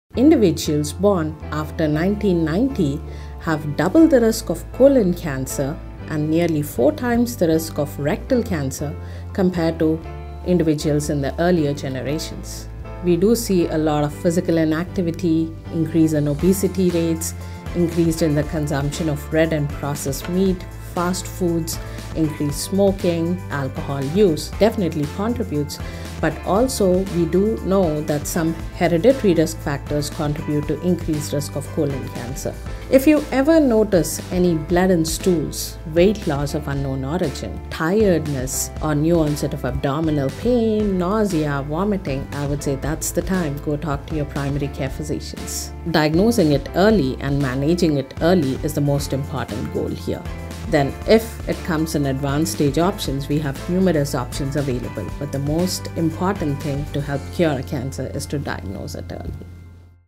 Colon Cancer Rates Increasing in Young Adults
Colon cancer rates are increasing in young adults. In this week's LiveWell Report - a closer look at the causes, symptoms and steps to take to help with prevention.